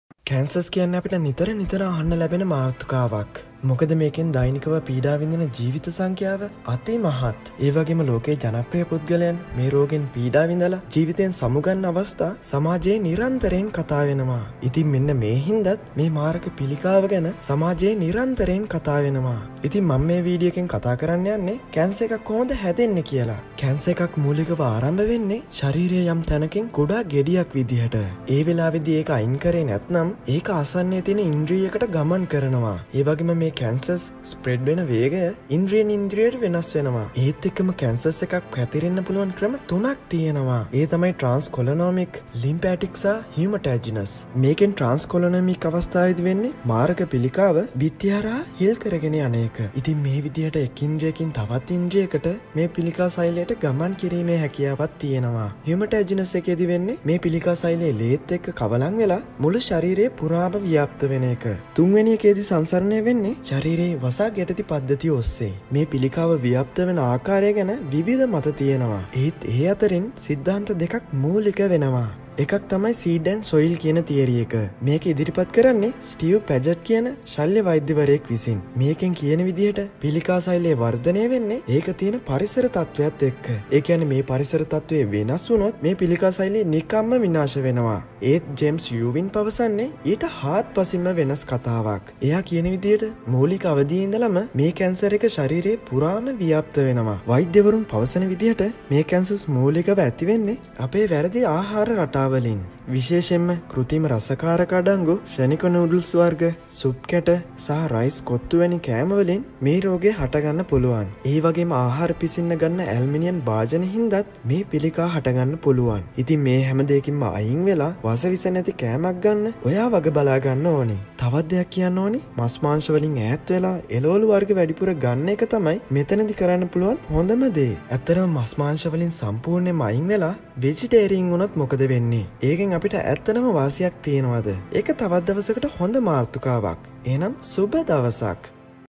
මාරක පිලිකාවේ ඇත්ත කතාව | How does cancer spread in Sinhala | DrBrain
Cancer is the name given to a collection of related diseases. In all types of cancer, some of the body's cells begin to divide without stopping and spread into surrounding tissues. Cancer can start almost anywhere in the human body, which is made up of trillions of cells.